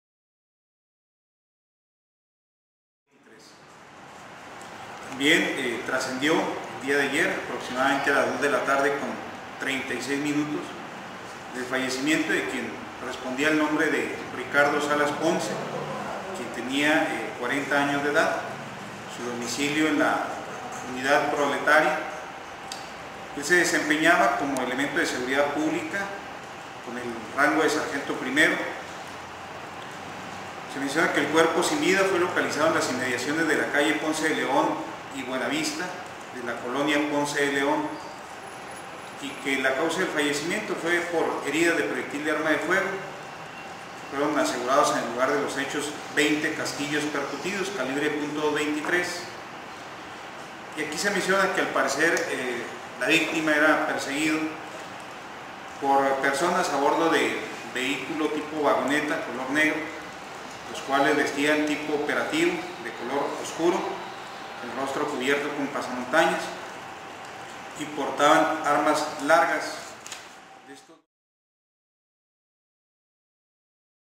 Ejecutan a elemento de seguridad pública después de ser per
Ejecutan a elemento de seguridad pública después de ser perseguido en la colonia Ponce de León